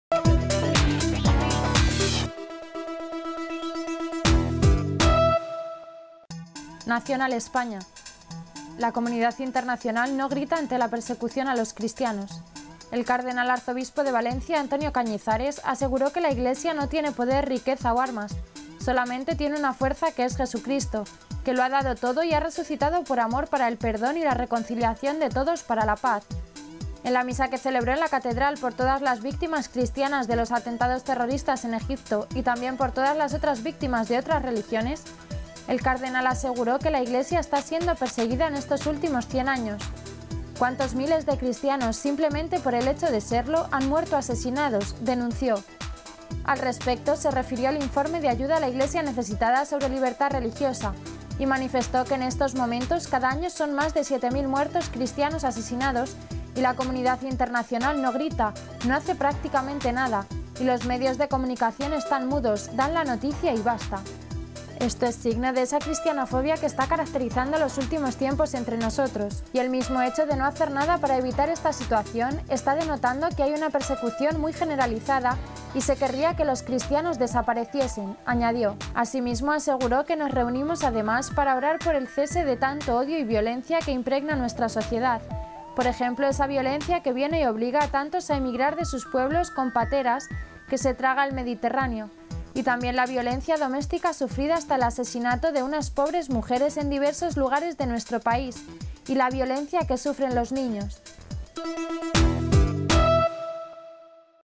Vídeo-Noticia: “La comunidad internacional no grita ante la persecución a los cristianos.”
El cardenal arzobispo de Valencia, Antonio Cañizares, aseguró que "la Iglesia no tiene poder, riqueza o armas, solamente tiene una fuerza que es Jesucristo que lo ha dado todo y ha resucitado por amor para el perdón y la reconciliación de todos para la paz".